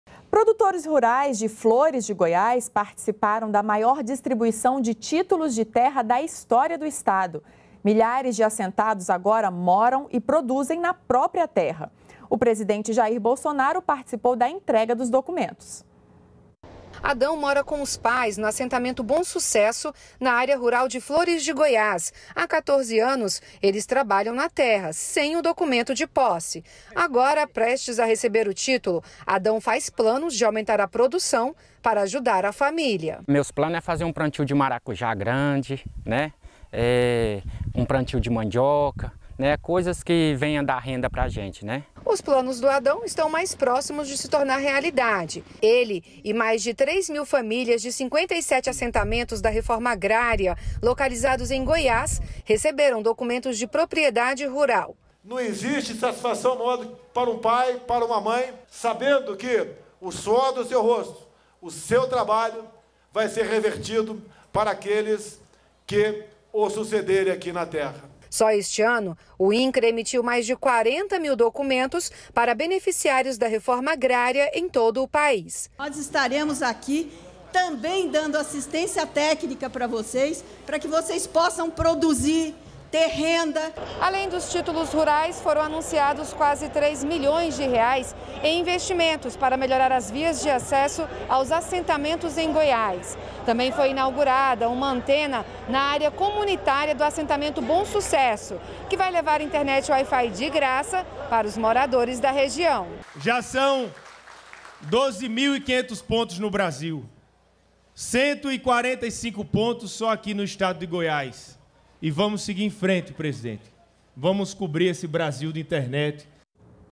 Presidente Bolsonaro entrega títulos rurais em Goiás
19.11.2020 - Produtores rurais de Flores de Goiás participaram da maior distribuição de títulos de terra da história do Estado. Milhares de assentados agora moram e produzem na própria terra. O presidente Jair Bolsonaro participou da entrega dos documentos.